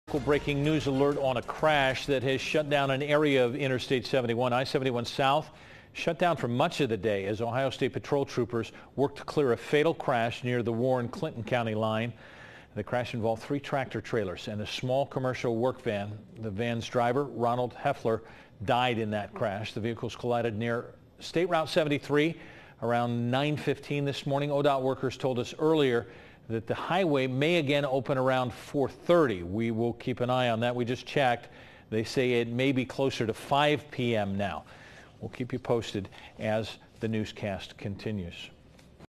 Fatal crash shuts down I-71 south in Warren Co.
WARREN COUNTY, Ohio (WKRC) -- I-71 south is shut down as the Ohio State Patrol investigates a fatal crash near the Warren/Clinton county line. OSP says the crash involved three tractor trailers and a small commercial work van. The van's driver, Ronald Hoeffler, was killed in the crash.  The vehicles collided on I-72 southbound near SR 73 at 9:20 on April 28. OSP says traffic was backing up due to a lane closure for the Jeremiah Morrow Bridge construction project. The OSP preliminary investigation finds Hoeffler was stopped behind one of the semis, when a second semi rear-ended Hoeffler's van and pushed off the right side of the road. That second semi then rear ended the back of the first before running into the median. A third semi swerved to avoid the crash but then hit both semis already involved in the crash. Traffic is being diverted to westbound SR 73 to southbound US 42 to eastbound SR 48 to I-71. Warren County officials say some semis are following cars to another detour involving SR 350. Tractor trailers are not allowed on that road and some are having difficulty navigating s-turns. The left lane of northbound I-71 was closed but has since reopened.ODOT says that the highway has reopened since the crash as of 4:30 p.m. on Tuesday.(Photo courtesy: Cynthia Colegrove) Follow us on Twitter @Local12  and LIKE us on Facebook  for updates!